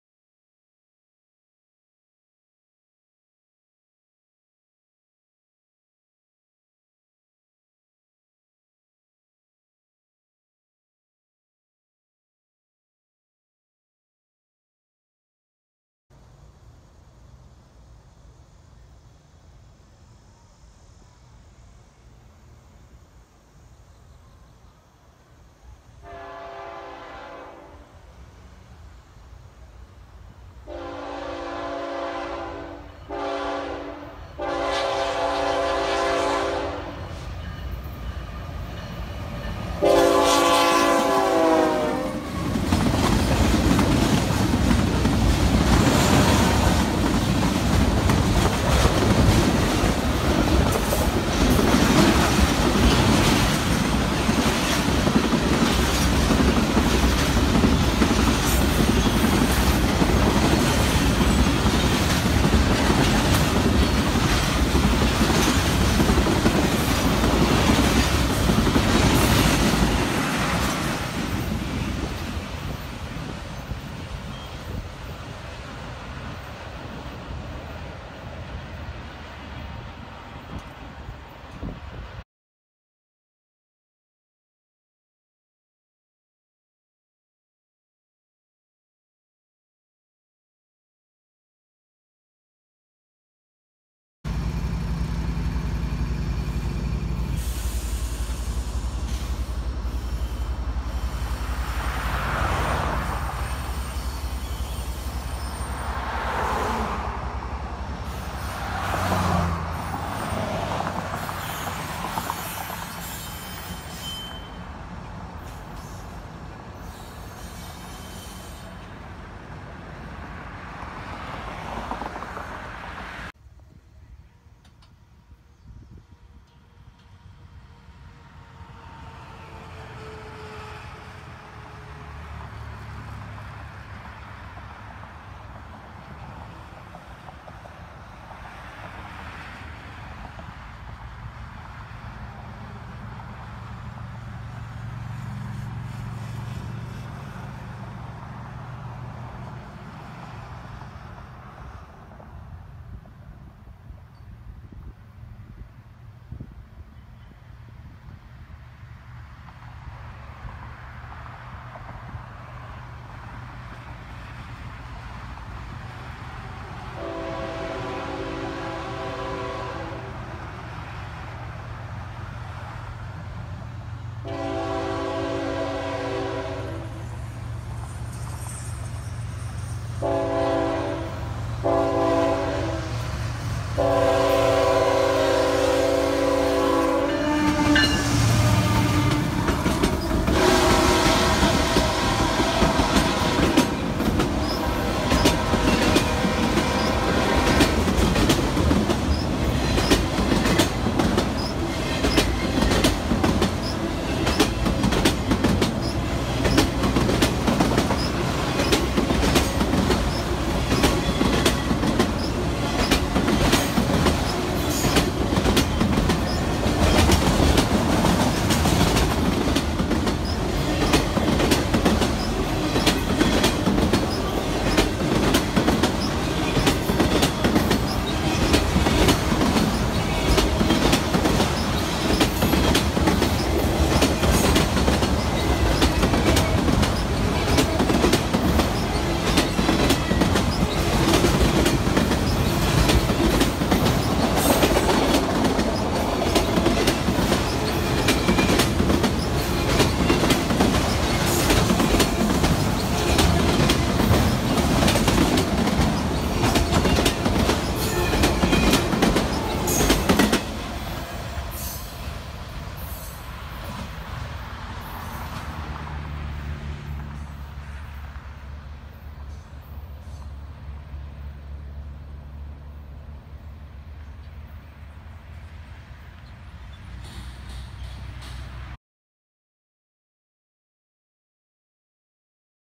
8/15/2017 Fortville & Ingalls, IN - CSX & UP power
I stopped at Fortville for a brief trackside visit, I caught a train of wind turbine parts powered by a single Union Pacific unit at track speed.  After a while, I headed home up IN-67.  As I passed thru Ingalls, I came upon a westbound auto rack train powered by 3 CSX units that was just starting after a full stop at CP 260 in Ingalls.  It crossed over from the 2 track to the 1 track that is generally reserved for eastbound traffic..